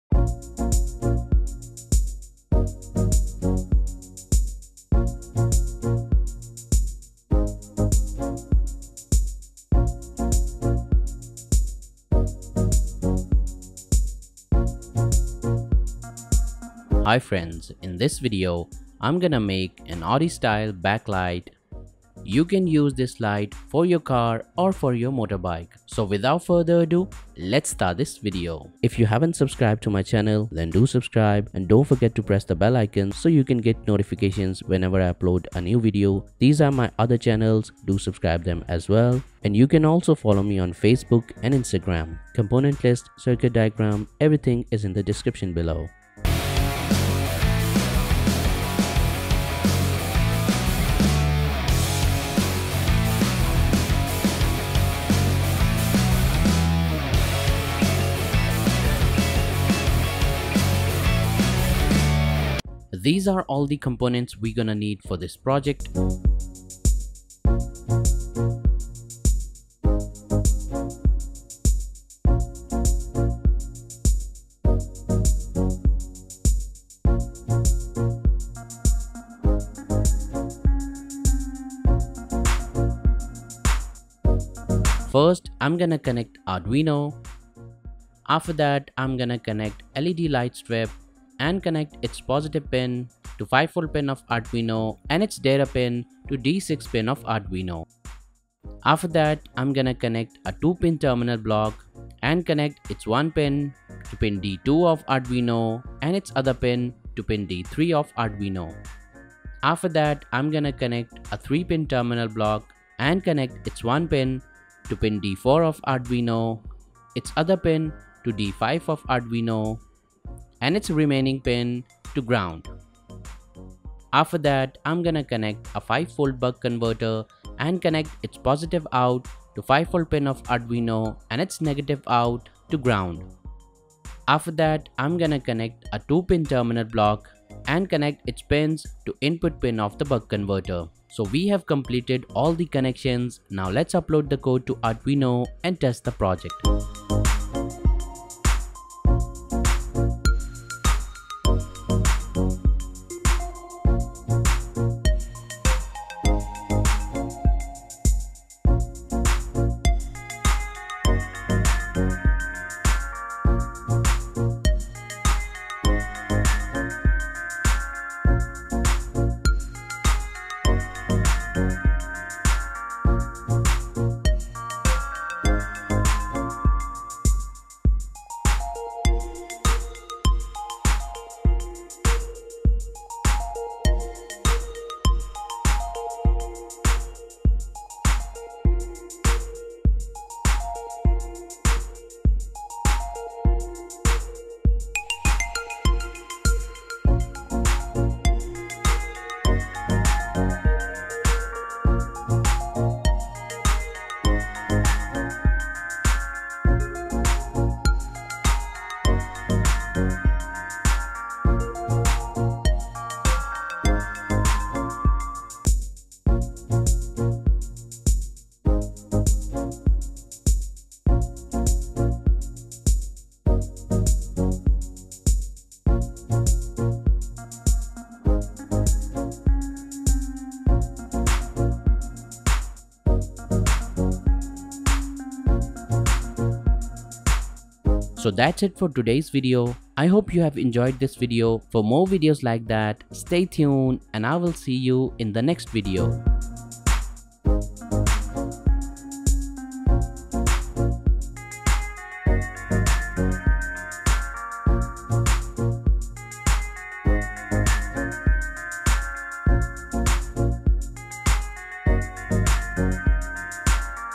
How To Make Audi Style Back Light | Sequential Tail Lights
Hi friends in this video I have made an Audi style tail light using Arduino.

Components:

1 X Arduino Nano

1x LM2596 5V Regulator

3x WS2812 Led Strip

1 x A4988 Module

2x 2 Pin Terminal Block

1x 3 Pin Terminal Block

jumper wires

3d printer